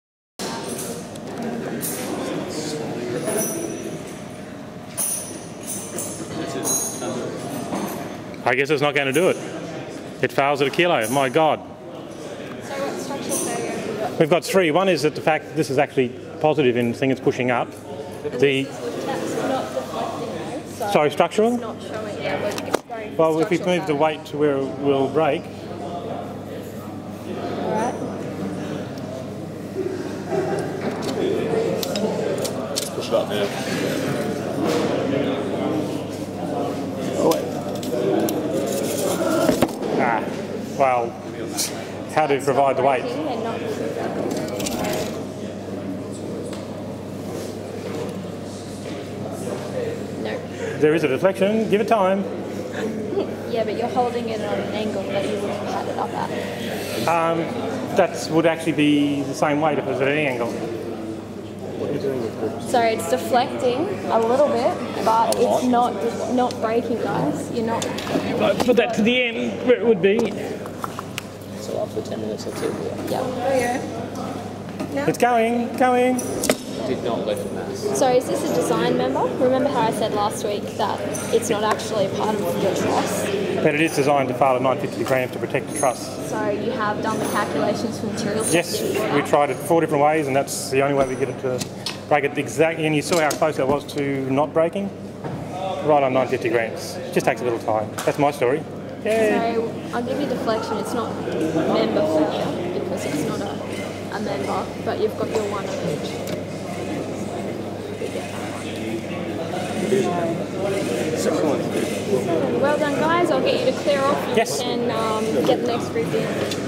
ENB 150 At the fail weight of 1kg
Balance issues cause fail, motor wont turn, rope doesn't grip and structure fails (: